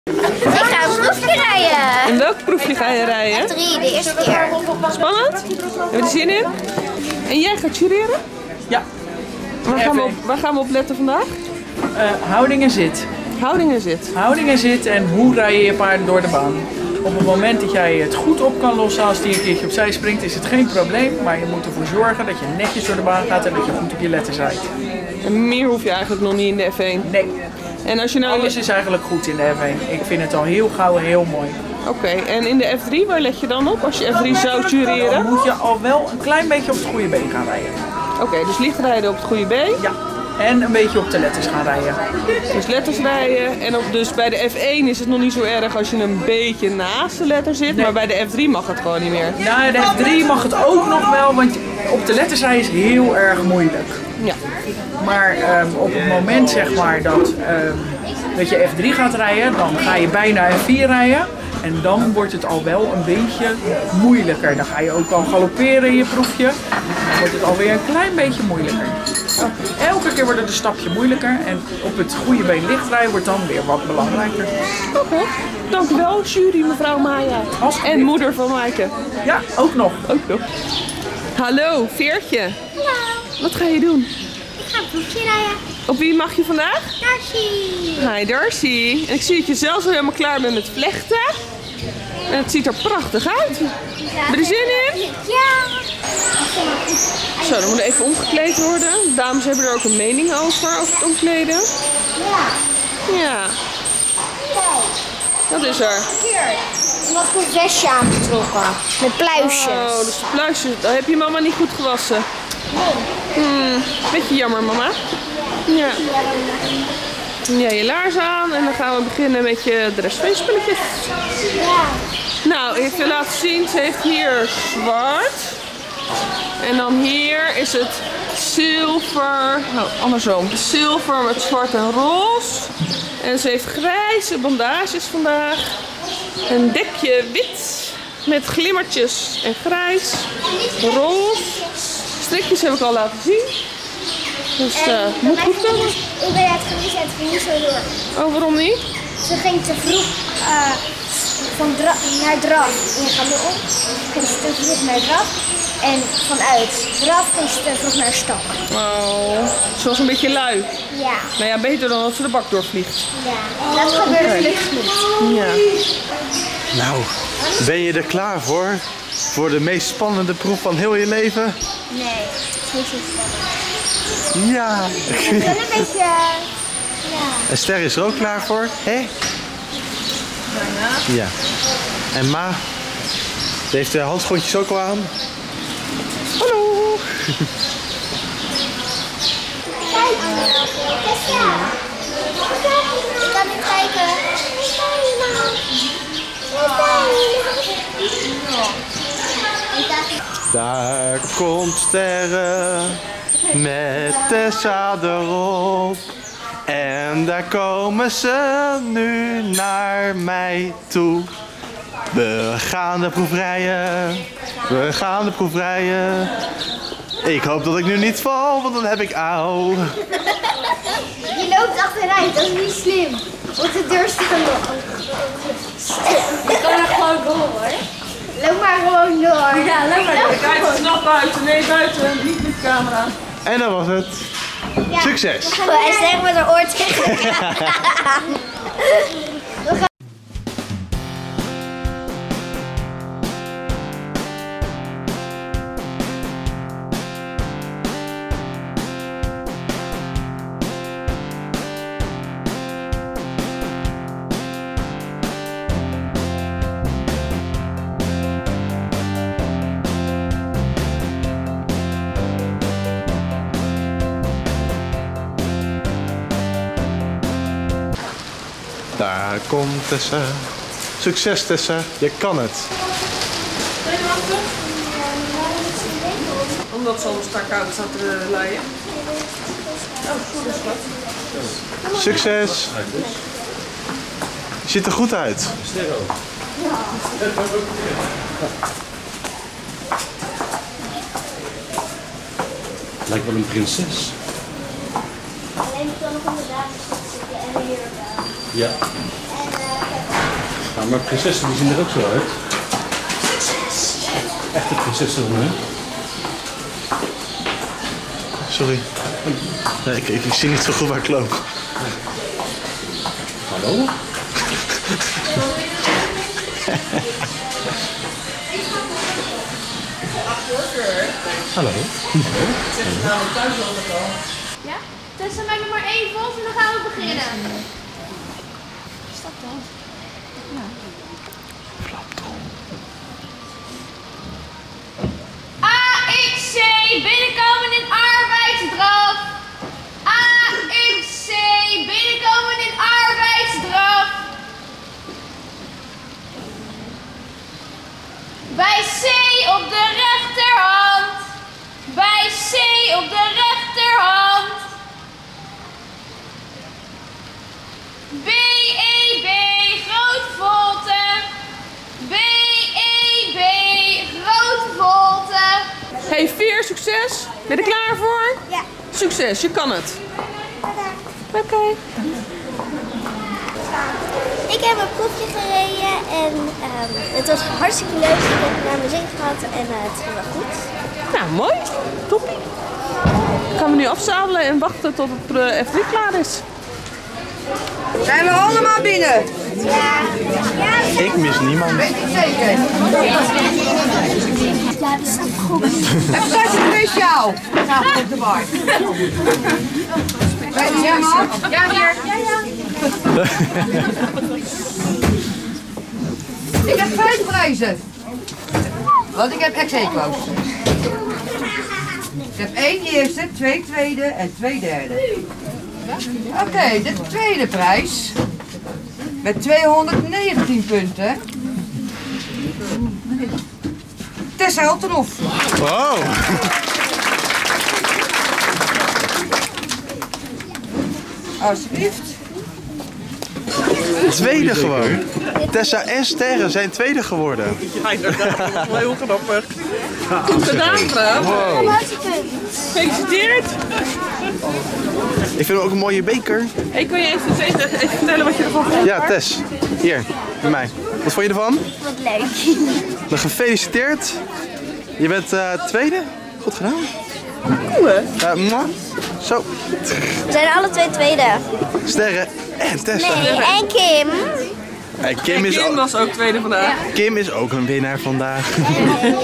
Tessa rijdt voor de 1e keer op haar pony Sterre de FNRS F3 |vlog| * Hart voor Paarden *
Tessa neemt je mee op haar wedstrijddag tijdens het rijden van de FNRS proefje F3. Ze rijdt  op haar pony Sterre.

Kijk de nieuwe weekvlog!: "Weekvlog 7 december 2019"